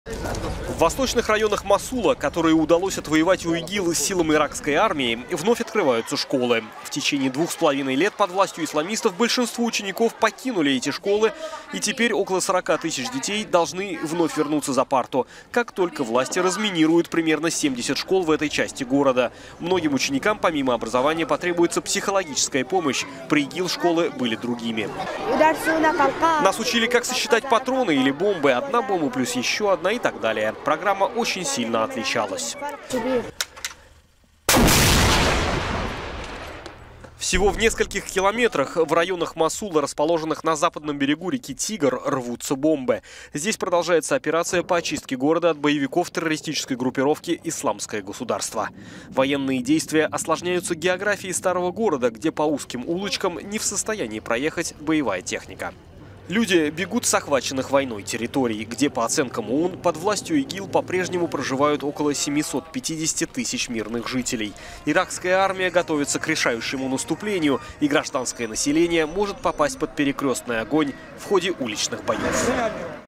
Половина Мосула освобождена, бои продолжаются
В восточных районах Мосула, которые удалось отвоевать у ИГИЛ силам иракской армии, вновь открываются школы. В течении 2,5 лет под властью исламистов большинство учеников покинули школы, и теперь около 40.000 детей должны вернуться за парту, как только власти разминируют примерно 70 школ в этой части города.